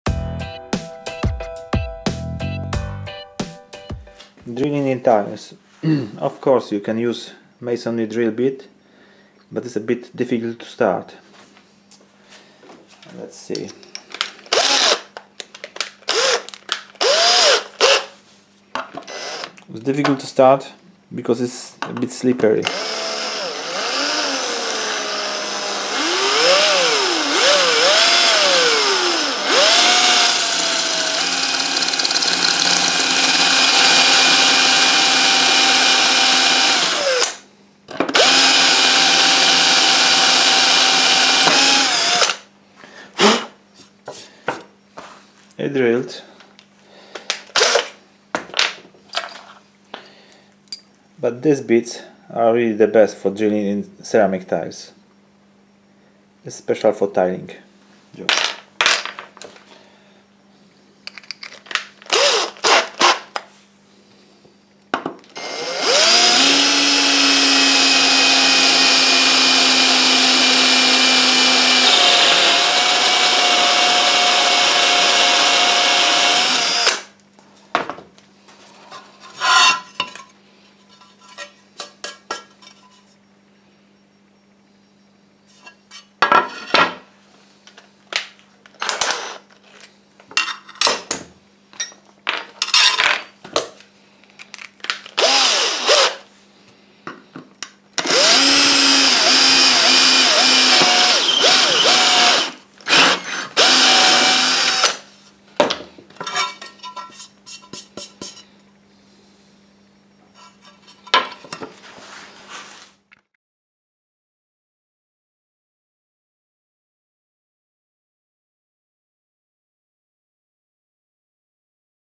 Drilling hole in ceramic tile. How to make a hole in a tile. What drill to use.
Hi . In this video I'd like to show you how to drill easy in ceramic tiles using proper tile drill bit. However when you drill in  a really tough tiles I recommend to cool down the drill bit in a cold  water from time to time.This will protect the drill bit.  Try to hold  the drill straight to the tile surface when drilling. Don't set  hammer mode when drilling.   Hope it will helps. Thanks